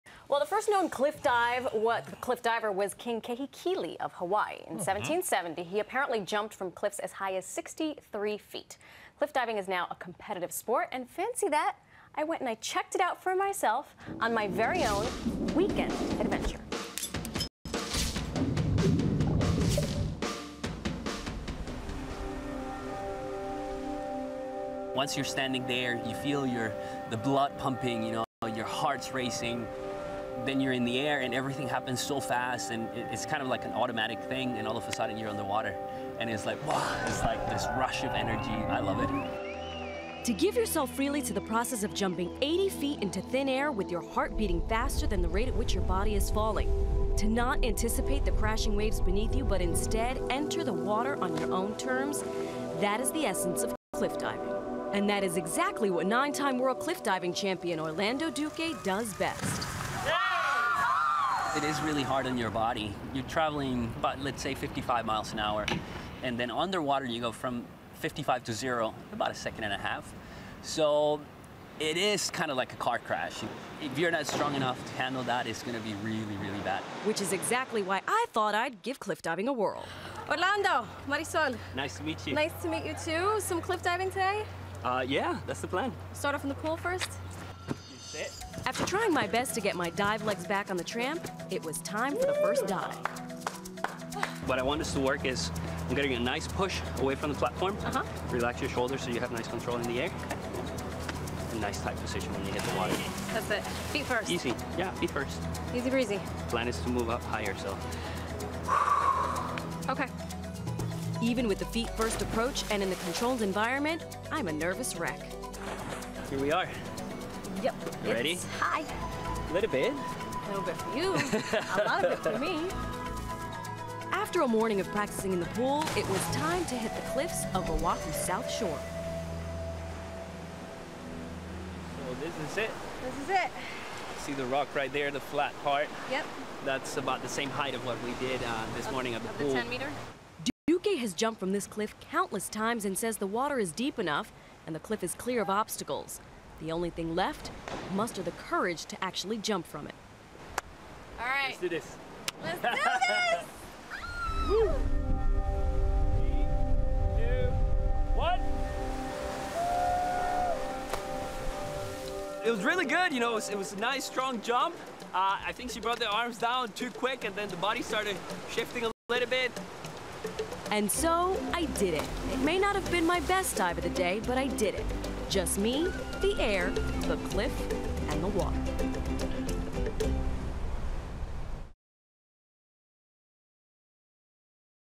Cliff Diving Off Hawaii's Rocky Coasts
Marysol Castro takes the plunge and tries the sport of Hawaiian warriors.